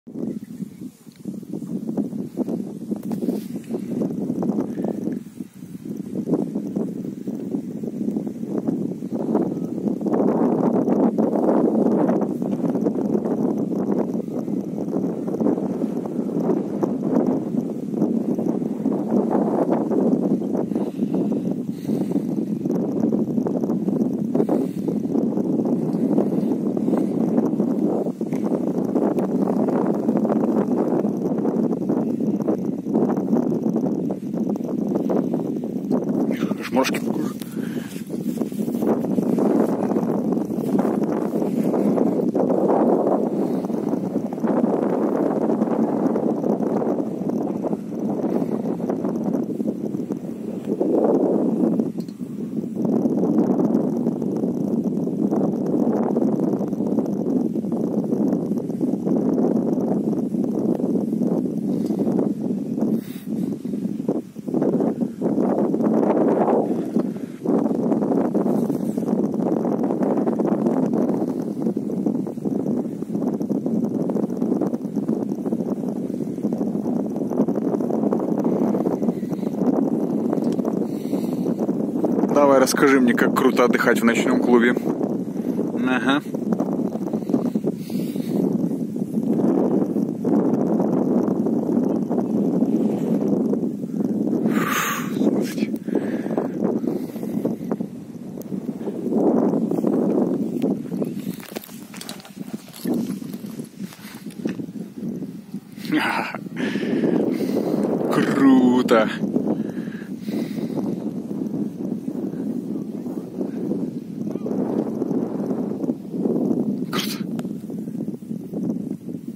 Бакота, над Днестром, бакотский залив, вершина, панорама и паропланы
При участии Jeep Renegade 2.4 Sport 4x4